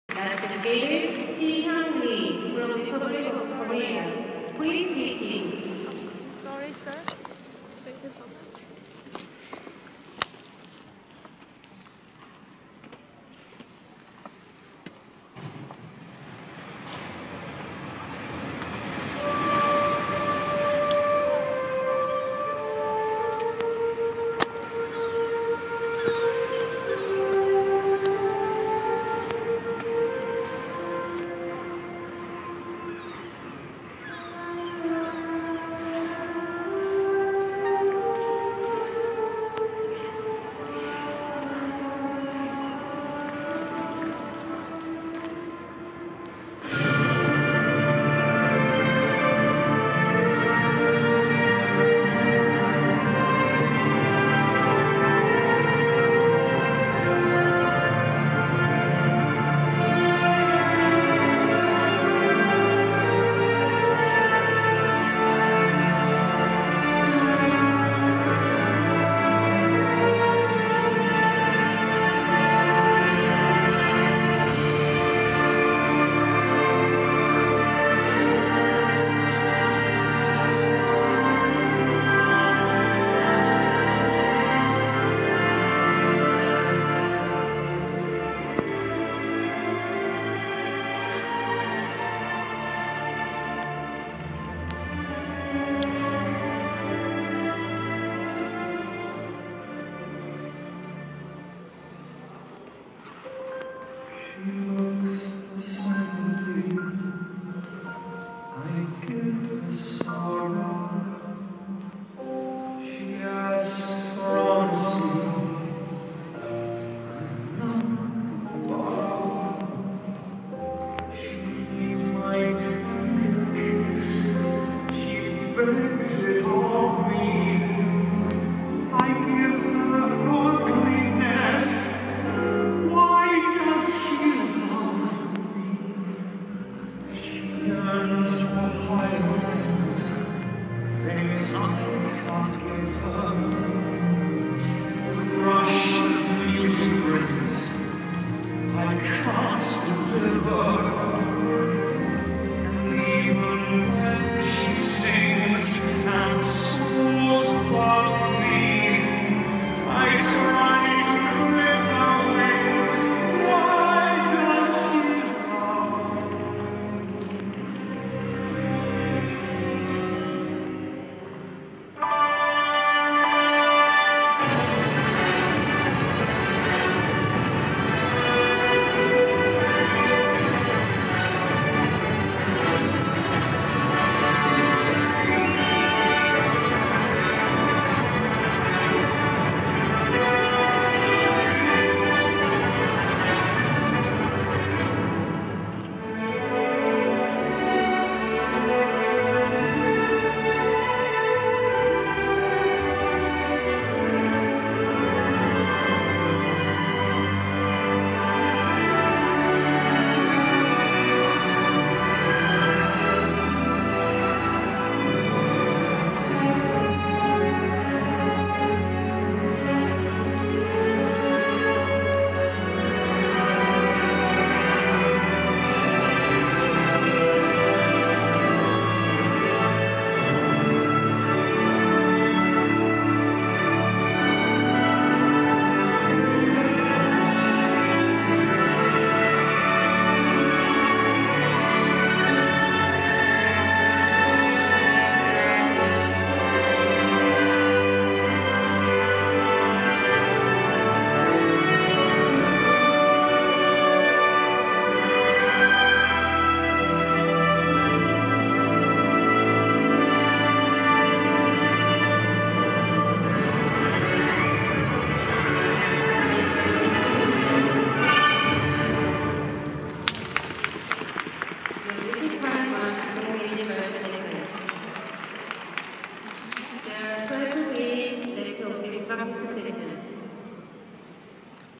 Sihyeong LEE (KOR)ㅣFSㅣPractice 3-6ㅣ2020 0204 4CC - Day1
ISU Four Continents Figure Skating Championships 2020
Feb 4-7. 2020. Mokdong Indoor Ice Rink, Seoul Republic of KOREA (South)ㅣ
Day1. Feb 4. Official Practice

Map.